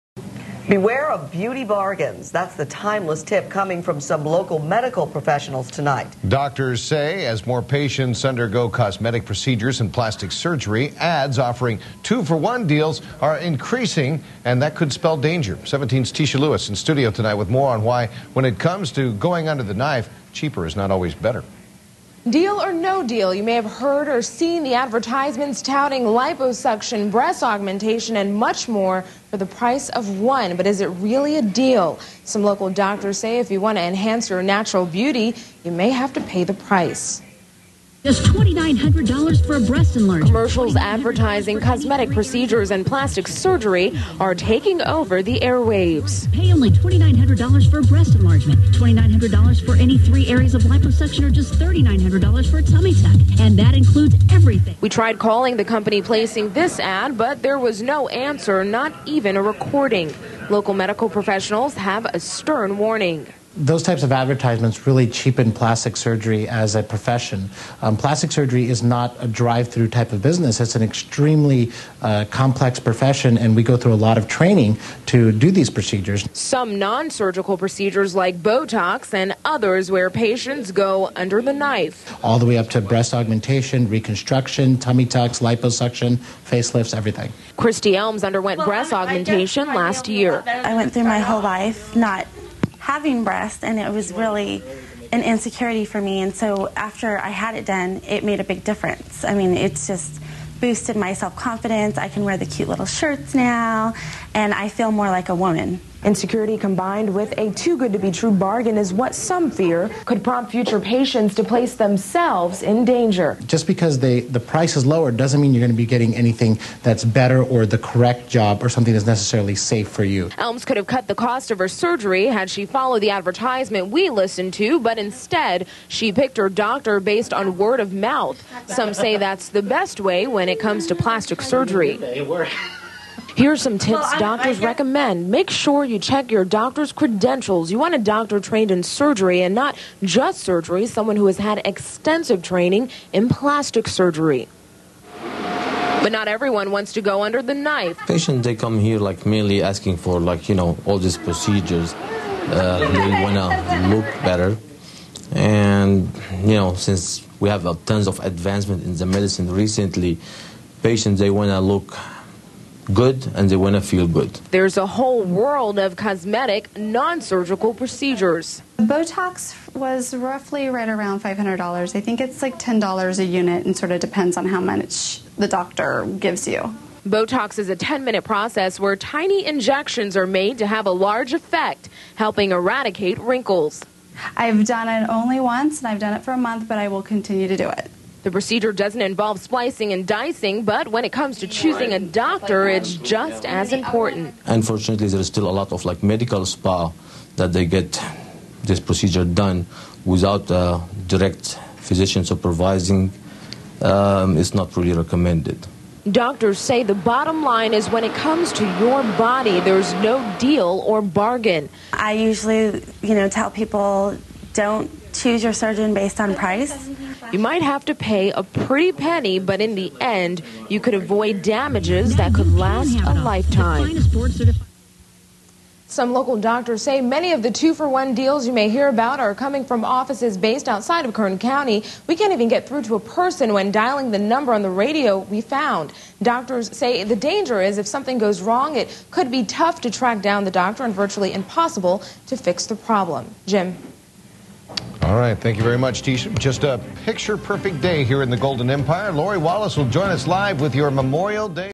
Interview with Dr. Darshan Shah - Channel 17 (KGET) Bakersfield Plastic Surgery
This interview with Dr. Darshan Shah M.D., F.A.C.S. focuses on the need to be wary of "Medical Beauty Bargains", and the importance of choosing a skilled & qualified cosmetic or laser surgeon.